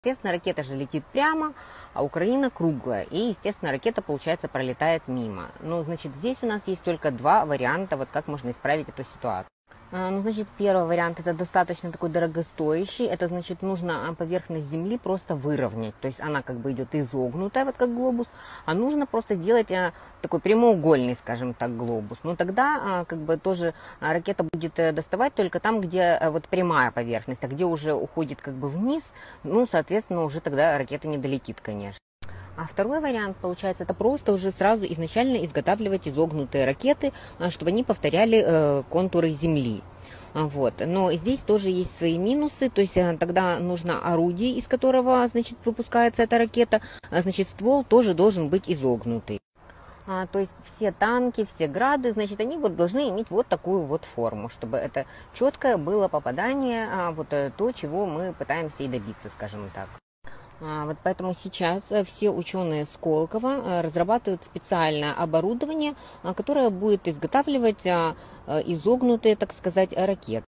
Ученица Арестовича и Гордона даёт вредные советы по ракетам
Ученица Арестовича и Гордона даёт вредные советы как увеличить точность ракет.
Короче, чтобы ракеты лучше попадали в наземные цели на Украине нужно:
Устранить закругление Земли путём подсыпки грунта, сделав глобус Украины параллелепипедом.
Перевооружить артиллерию, предоставив ей изогнутые ракеты, которые могли бы легче огибать земной контур.
Внимание!!! Смотреть видео, только предварительно убрав любый жидкости от клавиатуры!